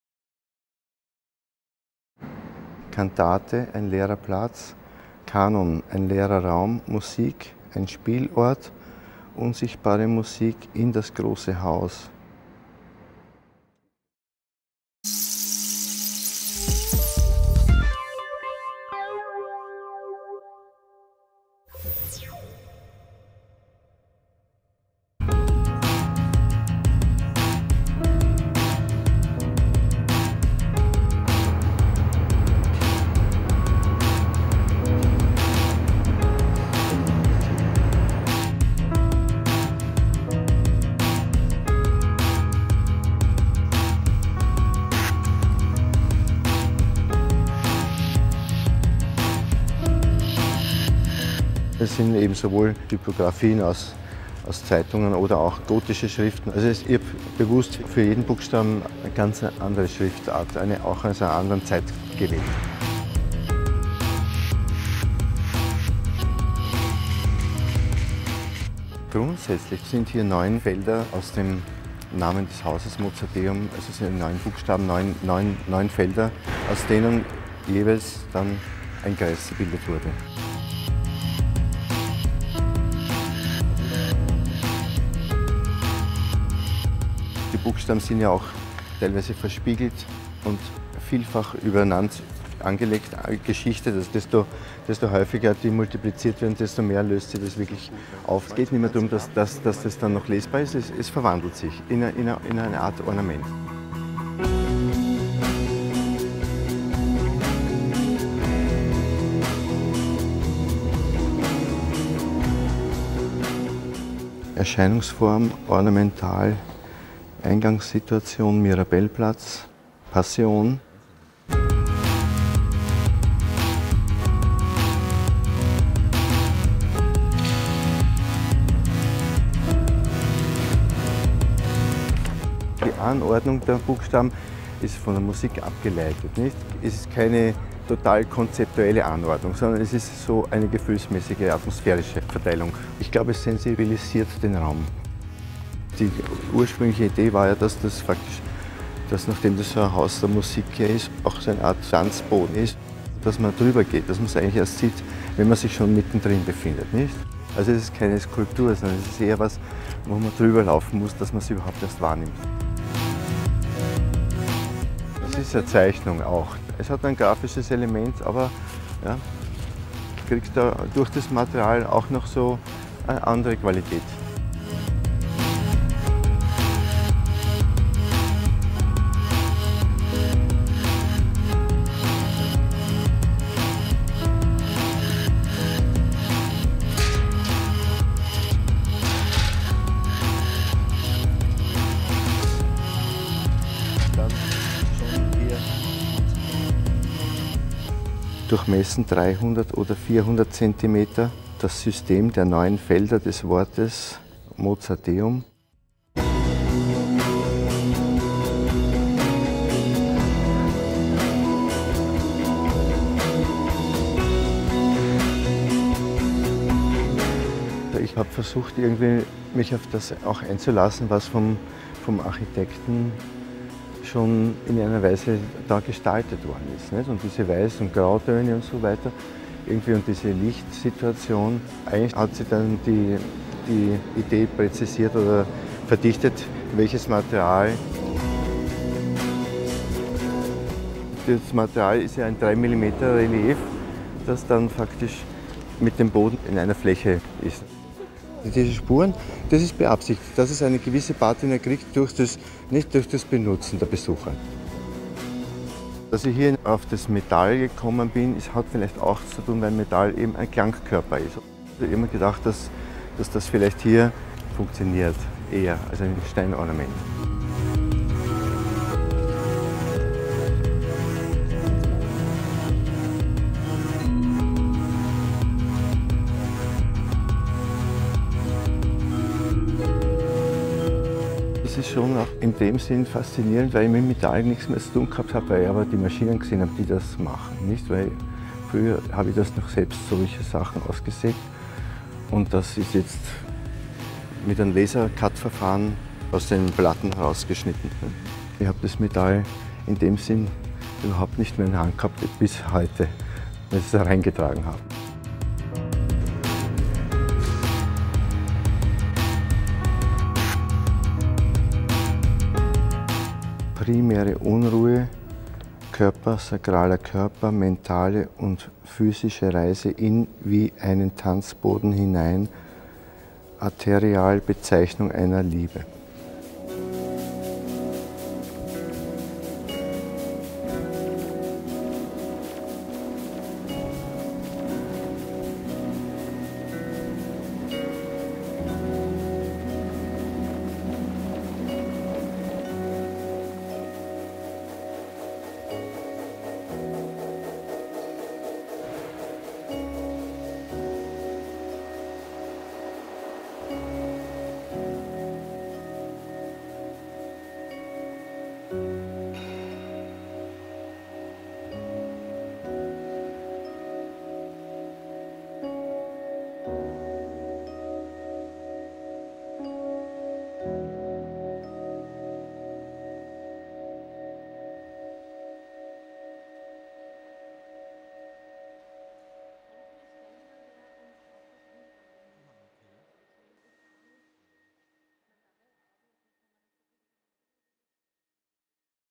BIG ART Kunst & Bau: Franz Graf "Hymnen, Tropen, Sequenzen"
Ein Kunst & Bau-Projekt von Franz Graf an der
Universität Mozarteum Salzburg im Auftrag von BIG Bundesimmobiliengesellschaft.